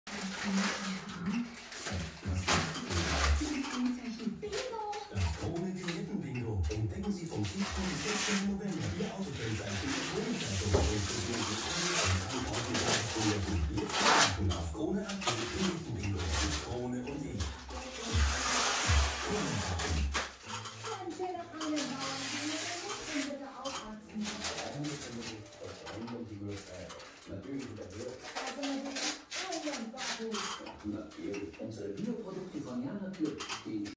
DIY Teamwork - Removing masking tape after painting the staircase and the upstairs corridor
This Video was taken after my wife and I finished painting the walls of the staircase and the corridor in our house.
Don't mind the sound in the background. it's Austrian radio channel Hitradio Ö3